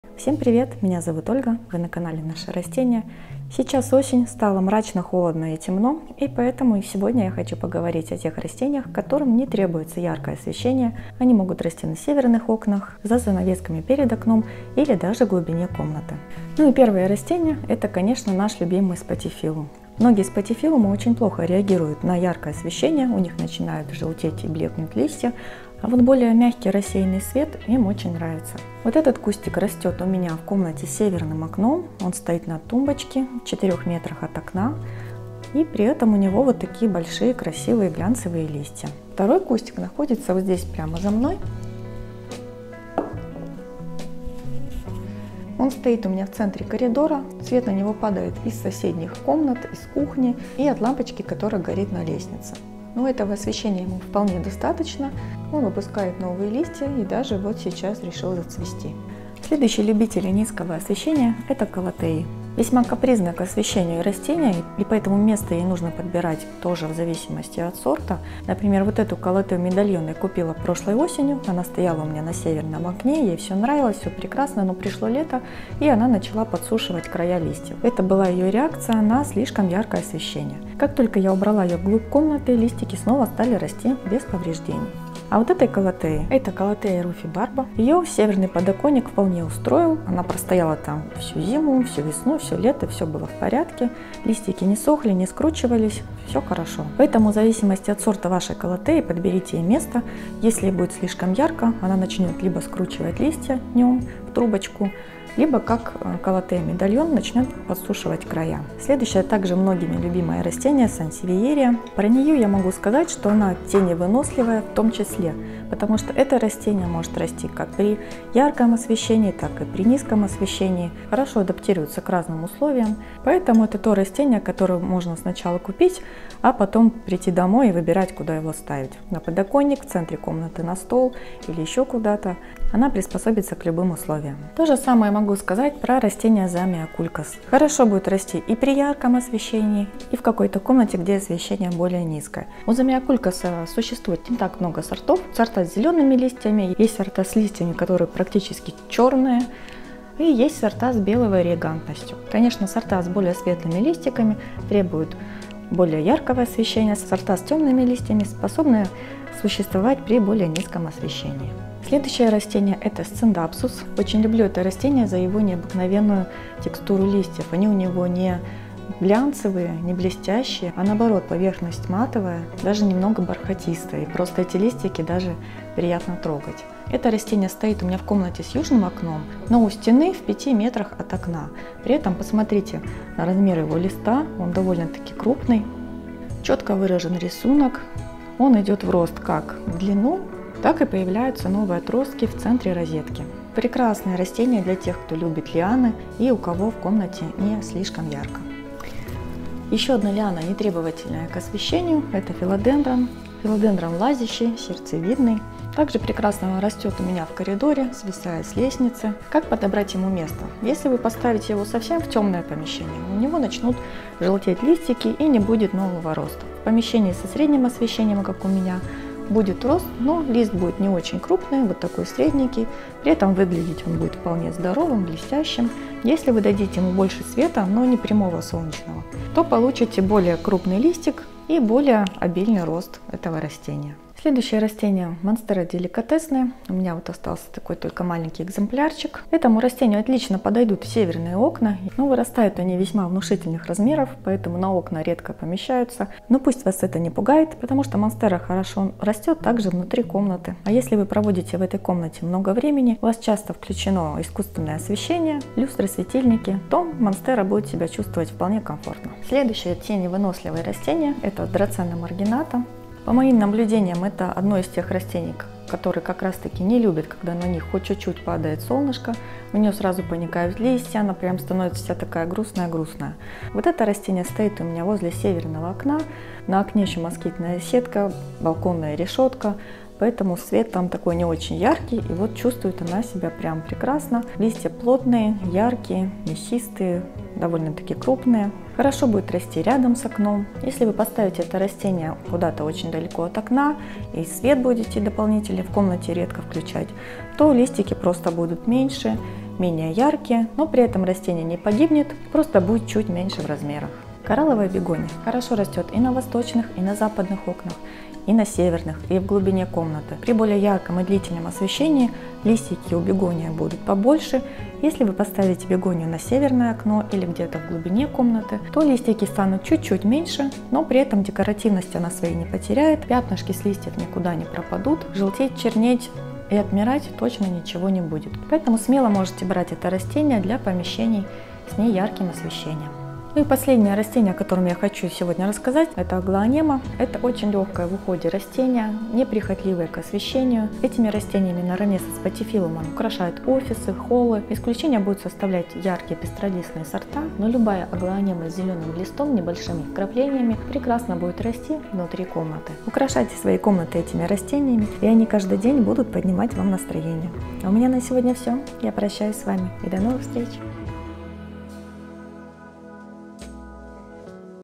№64. Растения, которые могут расти при слабом освещении.
В этом видео я расскажу о нескольких комнатных растениях, которые могут расти в помещении со слабым освещением.
1. Спатифиллум
2. Калатея
3. Сансевиерия
4. Замиокулькас
5. Сциндапсус
6. Филодендрон
7. Монстера
8. Драцена маргината
9. Бегония коралловая
10. Аглаонема

1. Группа Вконтакте "Наши растения"
2. Страница VK по продаже редких дорогих растений